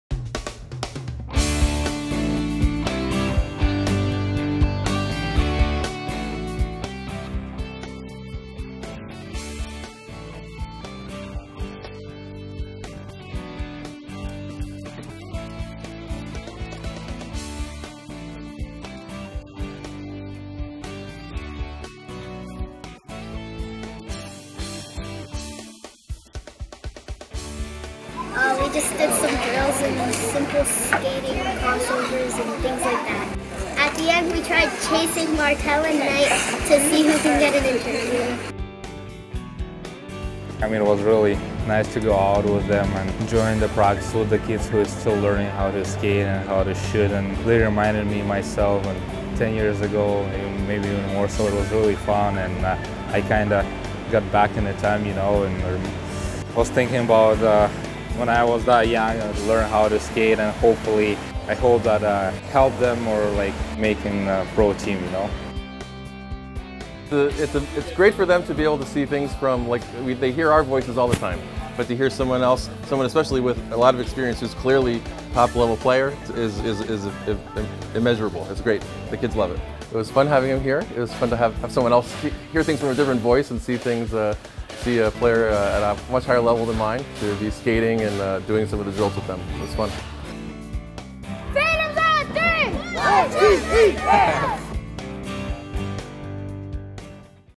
2016 Phantoms Youth Hockey Visits
Lehigh Valley Phantoms Danick Martel, Corban Knight, and Radel Fazleev join a couple of the Phantoms Youth practices at Steel Ice.